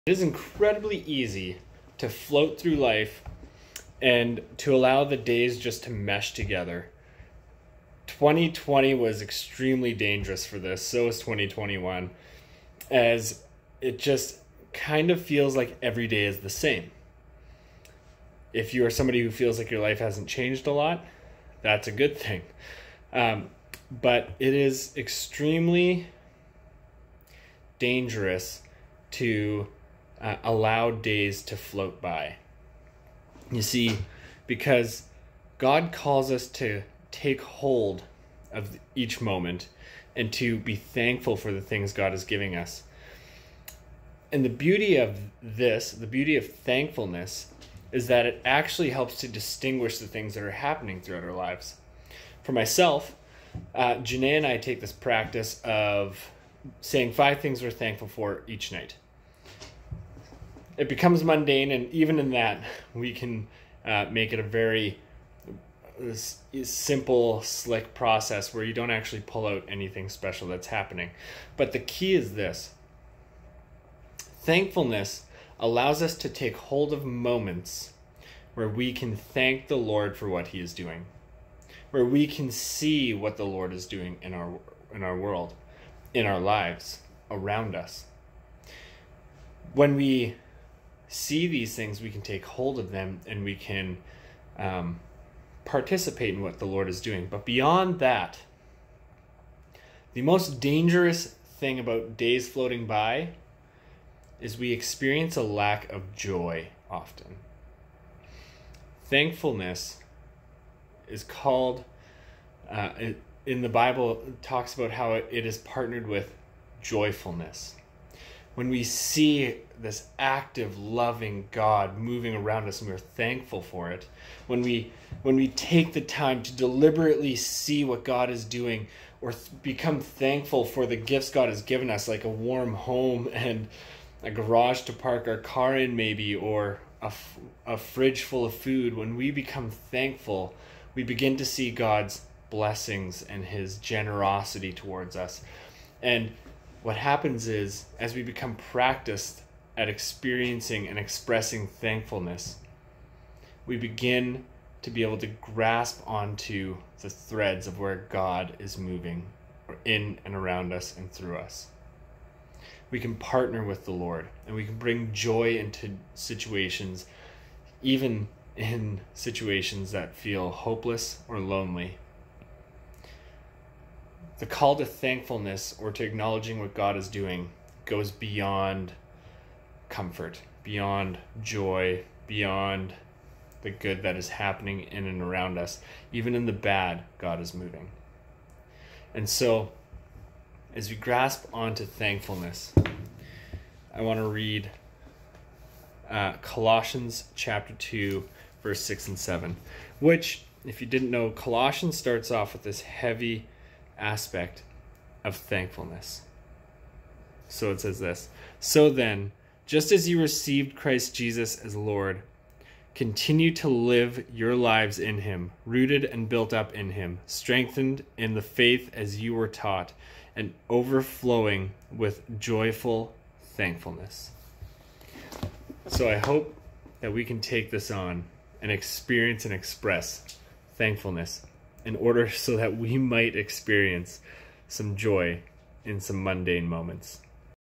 Finding Joy in Thanksgiving
Mark Wolfe reminds us that in the midst of mundane daily routines, it is important to remain thankful for the gifts our Father has given us. It brings joy to a mundane moment in life and allows us to see the goodness of God in and around us.

Colossians 2:6-7